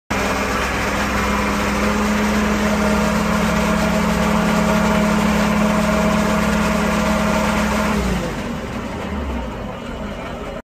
1974 GROVE RT60S For Sale
One owner 1974 Grove Cane with 3208 Cat engine, 3 speed transmission, turn table, 4 corner outriggers,18 ton lift capacity, and DRH block. Crane runs and operates as should, manual fuel shut off to shut off engine.  Contact Brian Studer with any questions or to schedule a time to come take a look.